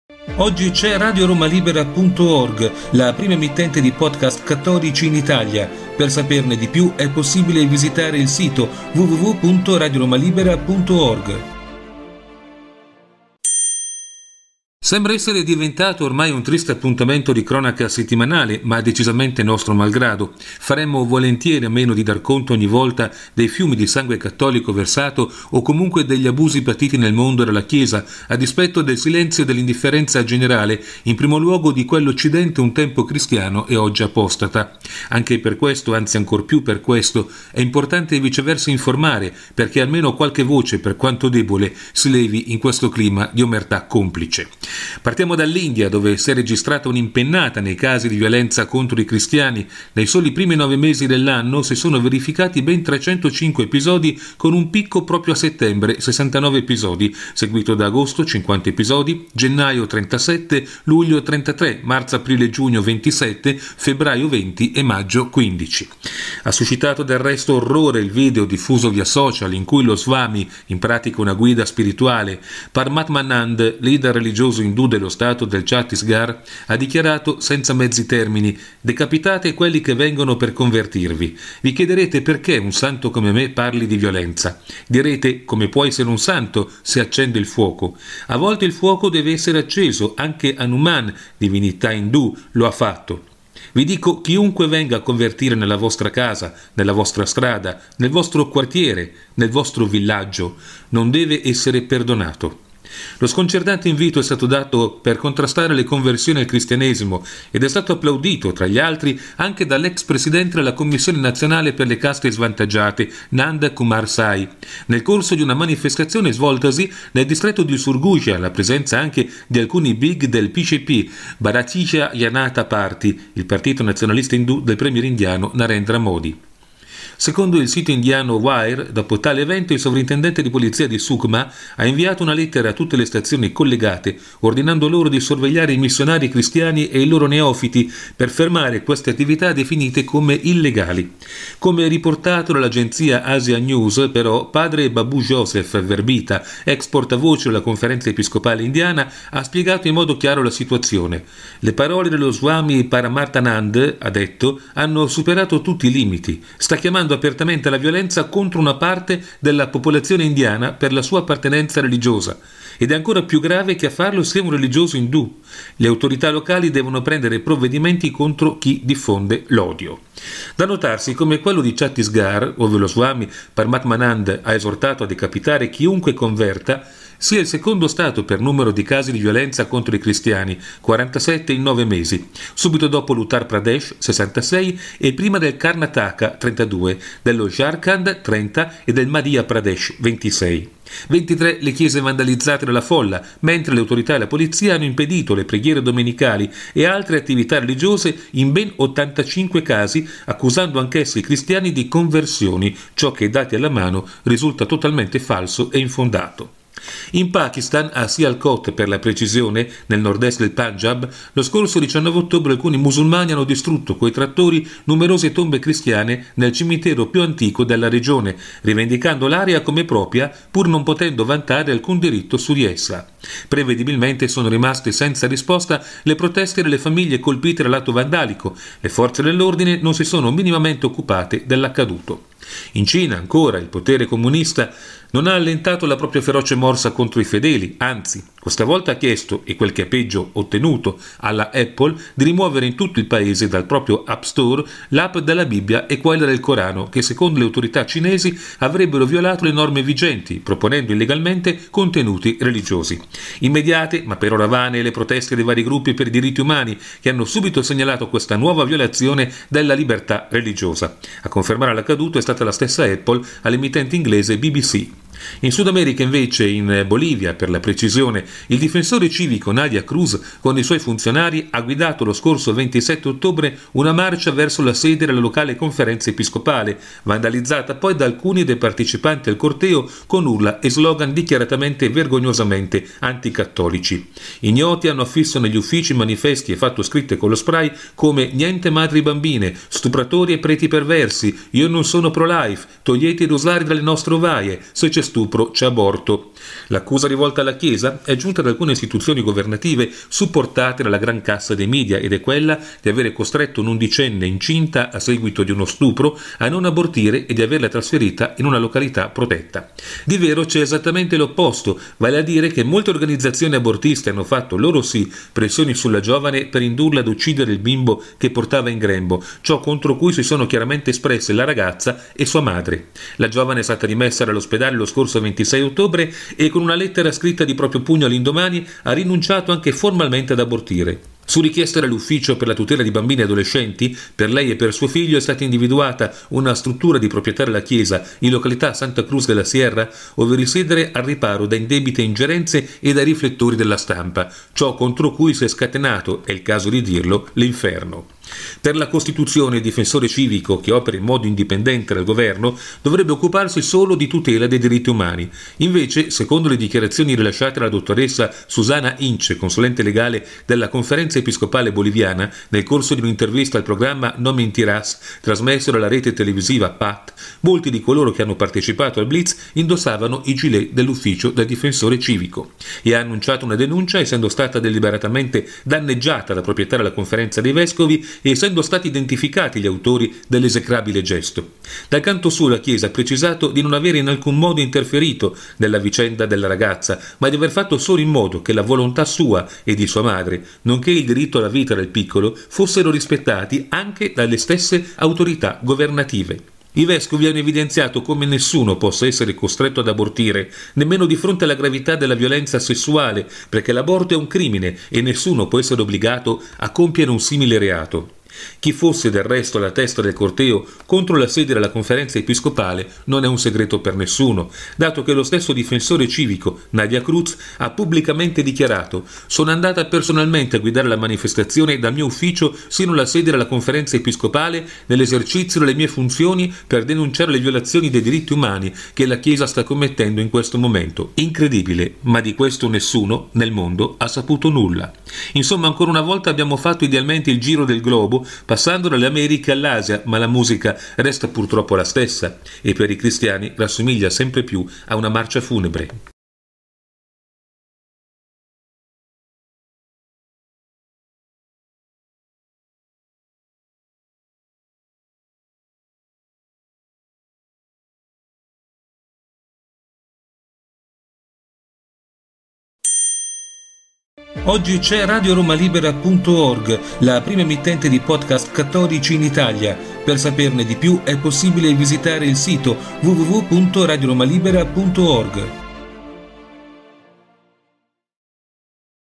1115 - Mauro Faverzani - Cristiani perseguitati, l’elenco si allunga ancora
Sembra esser diventato ormai un triste appuntamento di cronaca settimanale, ma decisamente nostro malgrado: faremmo volentieri a meno di dar conto, ogni volta, dei fiumi di sangue cattolico versato o comunque degli abusi patiti nel mondo dalla Chiesa, a dispetto del silenzio e dell’indifferenza generale, in primo luogo di quell’Occidente, un tempo cristiano ed oggi apostata. Anche per questo, anzi ancor più per questo è importante viceversa informare, perché almeno qualche voce, per quanto debole, si levi in questo clima di omertà complice.